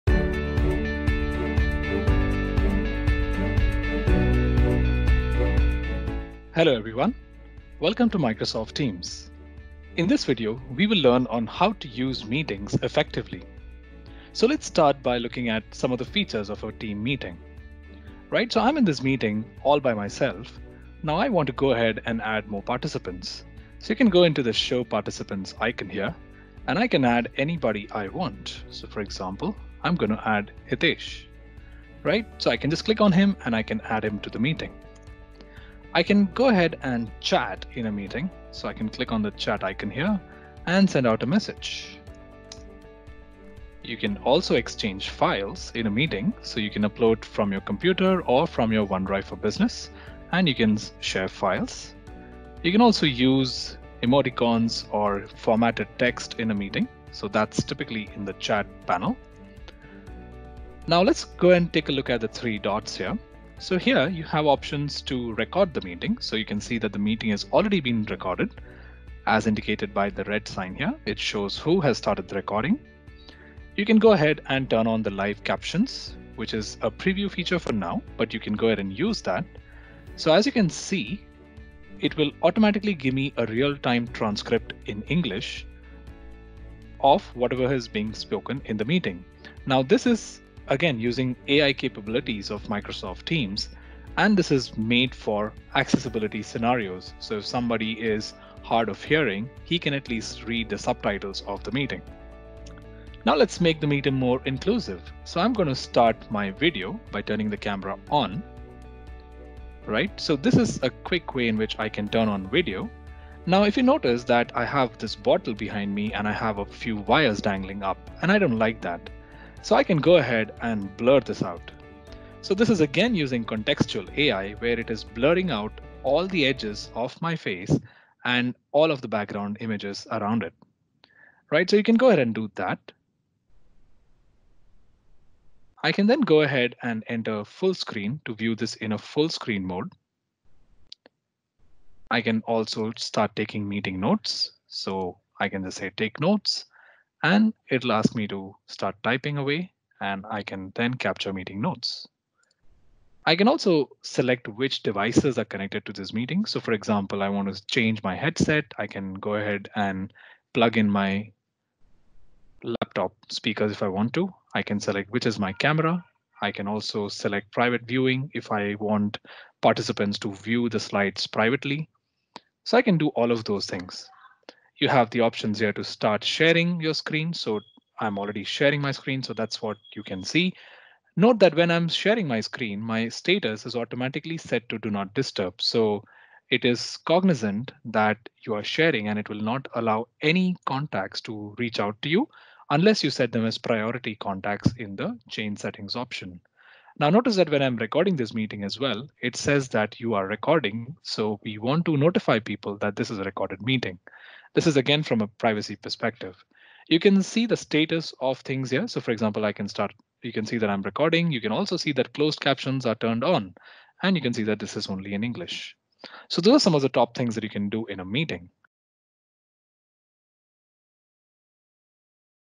How to effectively use all features of a Teams Meeting
Learn how to effectively use all features of a Teams Meeting such as Recording, Live Captions, Background Blur, etc.